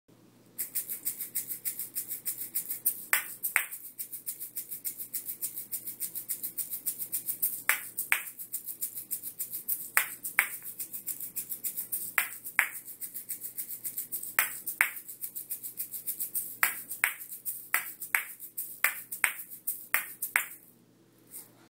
Kosika/Asalato/Patica Lesson 3
Shake the patica. Release on a downstroke and allow the patica to click on the topside and return to the bottom and click again. Catch the returning patica with the pinky and ring finger as they click. Remember the rope stays over the thumb on the topside click.